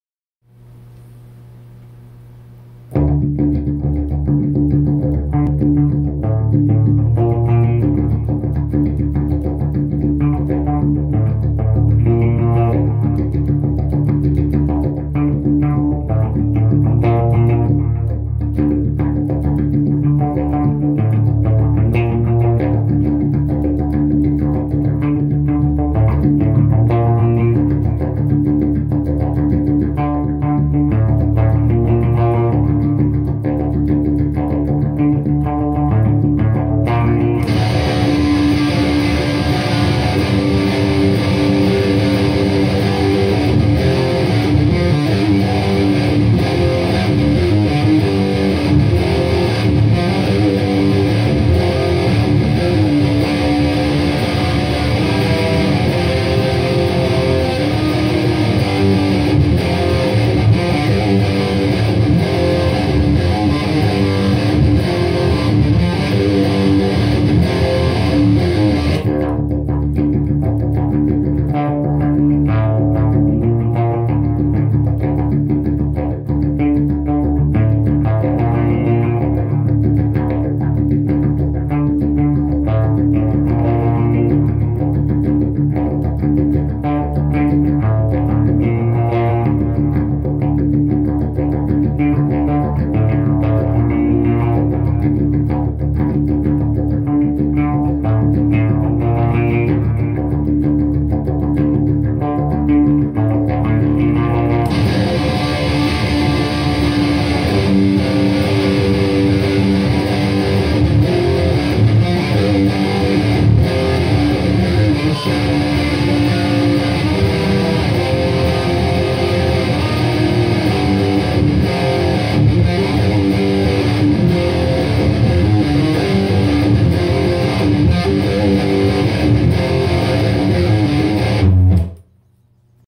Smobs
Messing around on a friends guitar before I fix it up for him